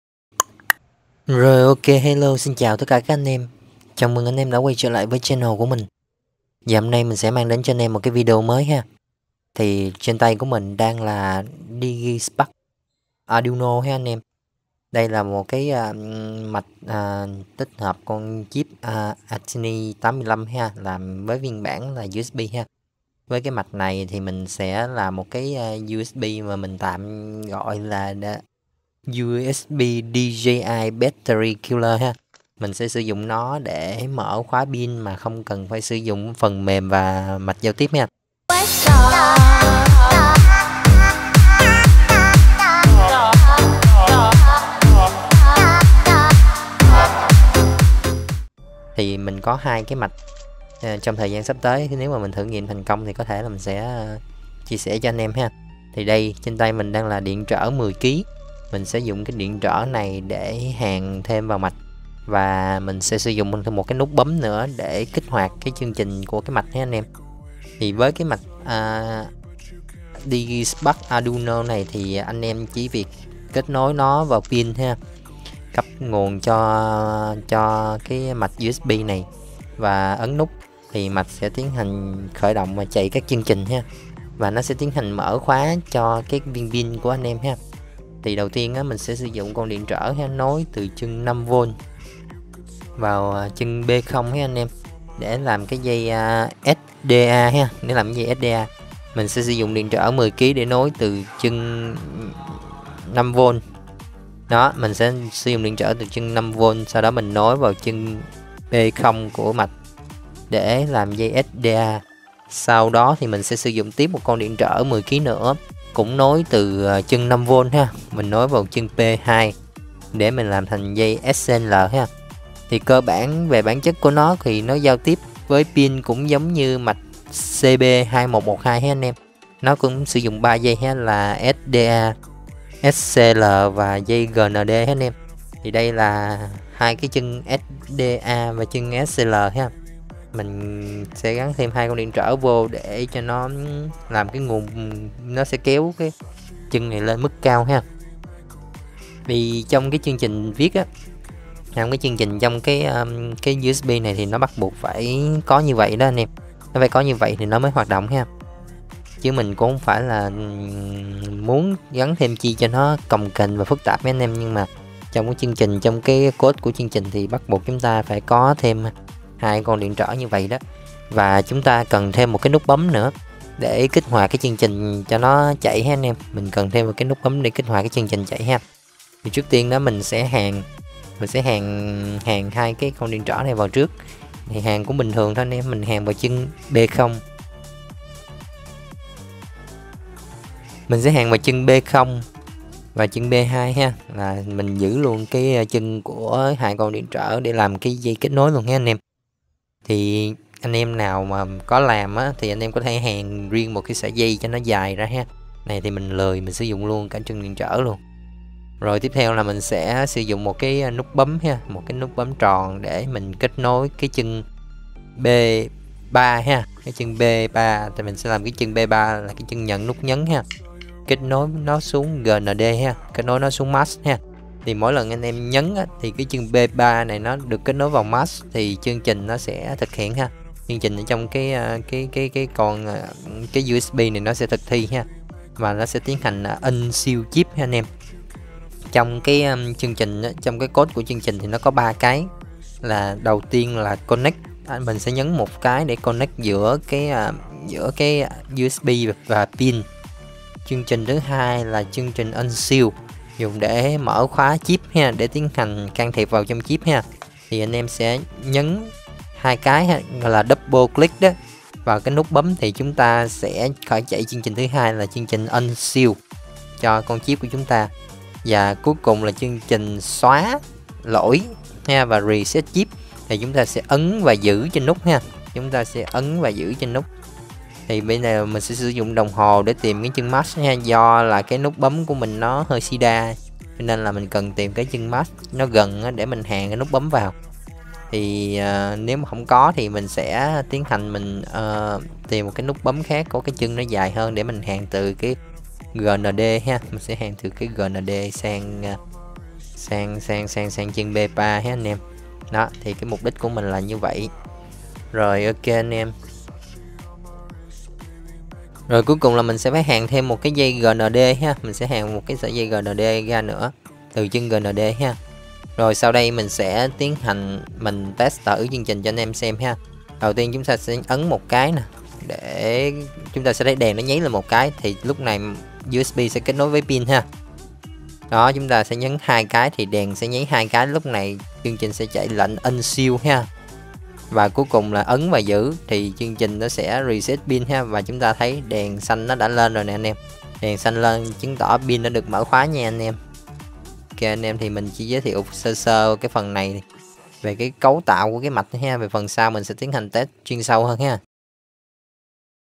Mở khóa pin DJI bằng "USB" và nút nhấn duy nhất, không cần phần mềm, không cần kiến thức, nhanh gọn
Hello mọi người, hôm nay mình sẽ mang đến mọi người 1 video mới

Nếu như phần mềm và máy tính để mở khóa pin DJI khi thay cell hoặc pin ngủ do để lâu không dùng dẫn đến khóa mạch BMS hoặc pin gặp lỗi Battery cell error, ... mà mọi thứ có vẻ hơi phức tạp và mang tính kỹ thuật thì hôm nay mình sẽ giới thiệu với ae một chiếc USB chuyên dùng để mở khóa chỉ với 1 nút nhấn duy nhất.

chỉ cần cắm USB vào nguồn điện 5V (sạc dự phòng, sạc điện thoại,...) kết nối USB với pin bằng 3 chân: SDA, SCL, GND sau đó ấn nút ... và thế là pin hoạt động trở lại như bình thường. thật dễ dàng phải không?

Với USB, mọi người sẽ không cần mang ra quán để kích pin nữa cũng như thay cell bất chấp mọi rào cản mà không lo mạch khóa.

Các dòng pin đang hỗ trợ:
+ Mavic 3